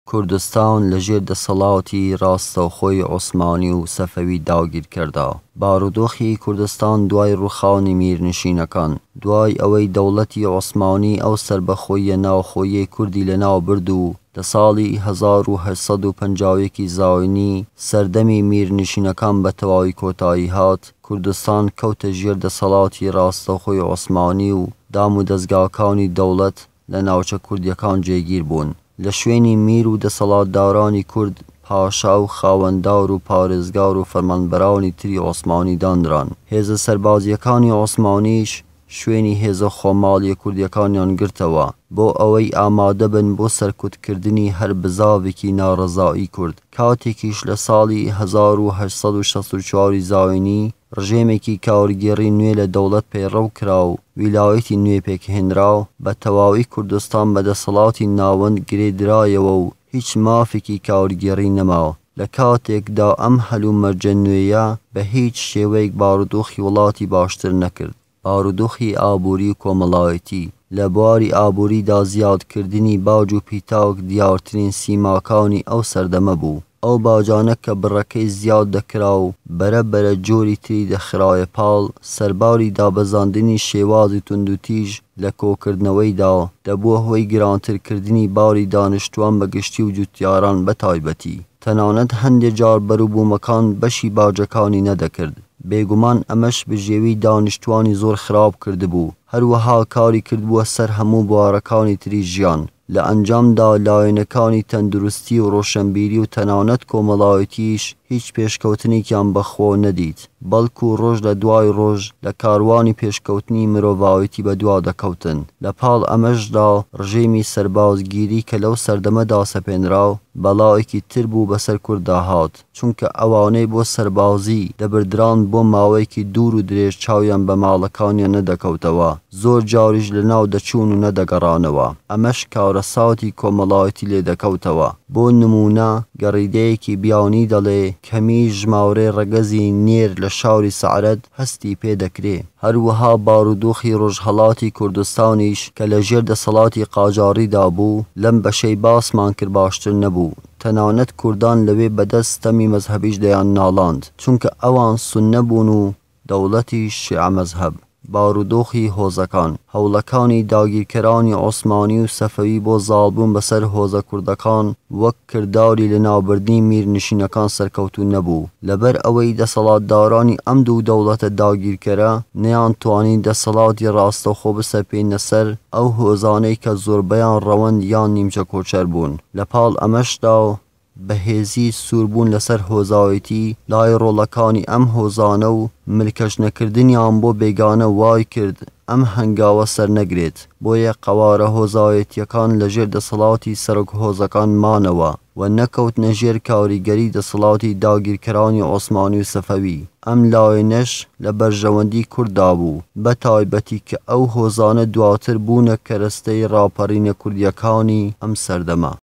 کوردستان لە ژێر دەسەڵاتی ڕاستەوخۆی عوسمانی و سەفەوی داگیرکەردا
بەشی سی و هەشتەمی مێژوو؛ کوردستان لە ژێر دەسەڵاتی ڕاستەوخۆی عوسمانی و سەفەوی داگیرکەردا:

بارودۆخی کوردستان دوای ڕووخانی میرنشینەکان:

دوای ئەوەی دەوڵەتی عوسمانی ئەو سەربەخۆییە ناوەخۆییەی کوردی لە ناوبرد و لە ساڵی ١٨٥١ی.ز سەردەمی میرنشینەکان بە تەواوی کۆتایی هات، کوردستان کەوتە ژێر دەسەڵاتی ڕاستەوخۆی عوسمانی و دام و دەزگاکانی دەوڵەت لە ناوچە کوردییەکان جێگیر بوون، لە شوێنی میر و دەسەڵاتدارانی کورد پاشا و خاوەندار و پارێزگار و فەرمانبەرانی تری عوسمانی دانران، هێزە سەربازییەکانی عوسمانیش شوێنی هێزە خوماڵییە کوردییەکانیان گرتەوە، بۆ ئەوەی ئامادەبن بۆ سەرکوتکردنی هەر بزاڤێکی ناڕەزایی کورد، کاتێکیش لە ساڵی ١٨٦٤ی.ز ڕژێمێکی کارگێڕی نوێ لە دەوڵەت پەیڕەو کرا و ویلایەتی نوێ پێکهێنرا، بە تەواوی کوردستان بە دەسەڵاتی ناوەند گرێدرایەوە و هیچ مافێکی کارگێڕی نەما، لە کاتێکدا ئەم هەلومەرجە نوێیە بە هیچ شێوەیەک بارودۆخی وڵاتی باشتر نەکرد.

بارودۆخی ئابووری و کۆمەڵایەتی:

لە بواری ئابووریدا زیادکردنی باج و پیتاک دیارترین سیماکانی ئەو سەردەمە بوو، ئەو باجانە کە بڕەکەی زیاد دەکرا و بەرەبەرە جۆری تری دەخرایە پاڵ، سەرباری دابەزاندنی شێوازی توندوتیژ لە کۆکردنەوەیدا، دەبووە هۆی گرانتر کردنی باری دانیشتووان بە گشتی و جوتیاران بەتایبەتی، تەنانەت هەندێ جار بەروبوومەکان بەشی باجەکانی نەدەکرد، بێگومان ئەمەش بژێوی دانیشتوانی زۆر خراپ کردبوو، هەروەها کاری کردبووە سەر هەموو بوارەکانی تری ژیان. لە ئەنجامدا لایەنەکانی تەندروستی و ڕۆشنبیری و تەنانەت کۆمەڵایەتیش هیچ پێشکەوتنێکیان بە خۆوە نەدیت، بەڵکوو ڕۆژ لە دوای ڕۆژ لە کاروانی پێشکەوتنی مرۆڤایەتی بە دوا دەکەوتن،
لە پاڵ ئەمەشدا ڕژێمی (سەربازگیری)کە لەو سەردەمەدا سەپێنرا، بەڵایەکی تر بوو بە سەر کورددا هات، چونکە ئەوانەی بۆ سەربازی دەبردران بۆ ماوەیەکی دوور و درێژ چاویان بە ماڵەکانیان نەدەکەوتەوە زۆر جاریش لە ناو دەچوون و نەدەگەڕانەوە، ئەمەش کارەساتی کۆمەڵایەتی لێدەکەوتەوە، بۆ نموونە گەڕیدەیەکی بیانی دەڵێ: (کەمی ژمارەی ڕەگەزی نێر لە شاری “سەعەرد” هەستی پێدەکرێ).

هەروەها بارودۆخی رۆژهەڵاتی کوردستانیش کە لە ژێر دەسەڵاتی قاجاری (ئێران)دا بوو لەم بەشەی باسمان کرد باشتر نەبوو، تەنانەت کوردان لەوێ بەدەست ستەمی مەزهەبیش دەیانناڵاند، چونکە ئەوان سوننە بوون و دەوڵەتیش شێعە مەزهەب.

بارودۆخی هۆزەکان:

هەوڵەکانی داگیرکەرانی عوسمانی و سەفەوی بۆ زاڵبوون بە سەر هۆزە کوردەکان وەک کرداری لە ناوبردنی میرنشینەکان سەرکەتوو نەبوو، لەبەر ئەوەی دەسەڵاتدارانی ئەم دوو دەوڵەتە داگیرکەرە نەیانتوانی دەسەڵاتی ڕاستەوخۆ بسەپێننە سەر ئەو هۆزانەی کە زۆربەیان ڕەوەند یان نیمچە کۆچەر بوون، لەپاڵ ئەمەشدا بەهێزی(سووربوون لەسەر هۆزایەتی) لای رۆڵەکانی ئەم هۆزانە و ملکەچ نەکردنیان بۆ بێگانە وای کرد ئەم هەنگاوە سەر نەگرێت، بۆیە قەوارە هۆزایەتییەکان لە ژێر دەسەڵاتی سەرۆک هۆزەکان مانەوە، وە نەکەوتنە ژێر کاریگەری دەسەڵاتی داگیرکەرانی عوسمانی و سەفەوی. ئەم لایەنەش لە بەرژەوەندی کورد دابوو بەتایبەتی کە ئەو هۆزانە دواتر بوونە کەرەستەی ڕاپەڕینە کوردیەکانی ئەم سەردەمە.